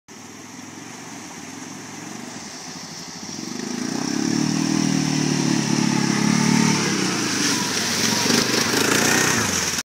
One more time
Jadon taking Mom for one more round.